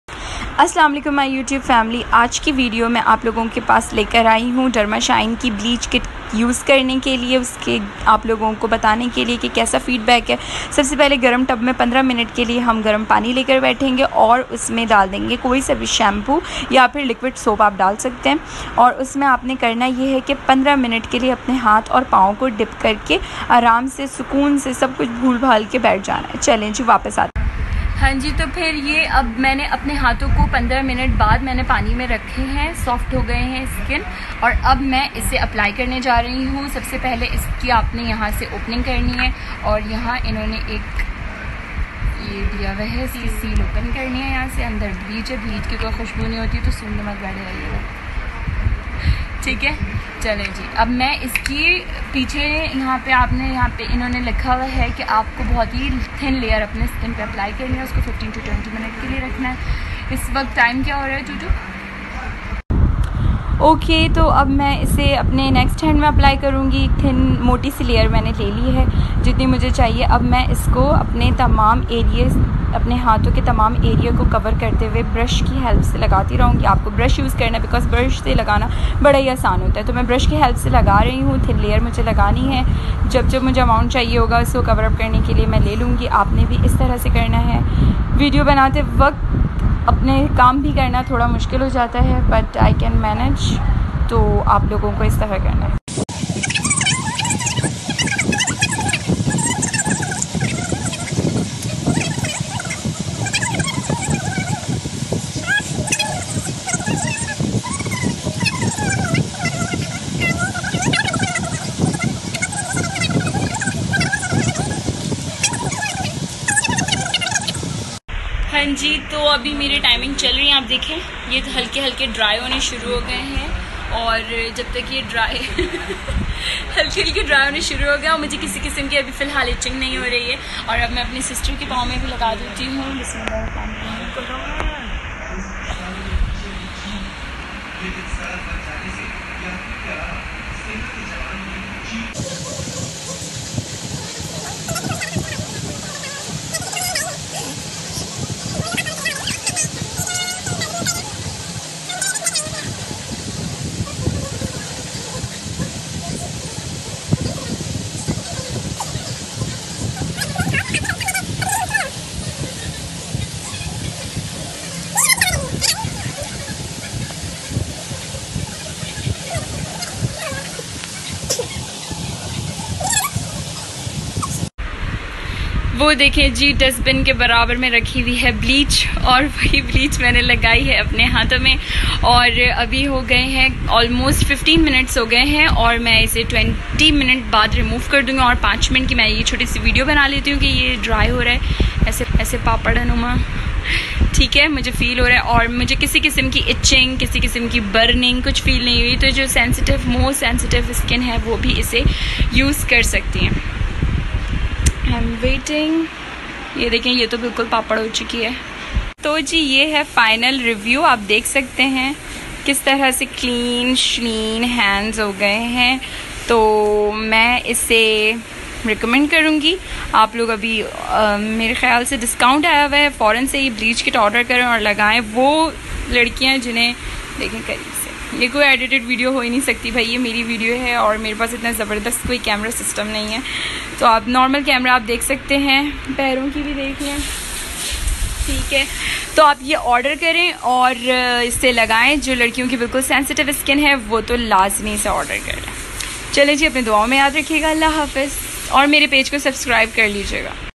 Derma shine hand and feet bleaching cream full guide step by step and review....
Easy and effective
With derma shine product
Kahkashan and tooba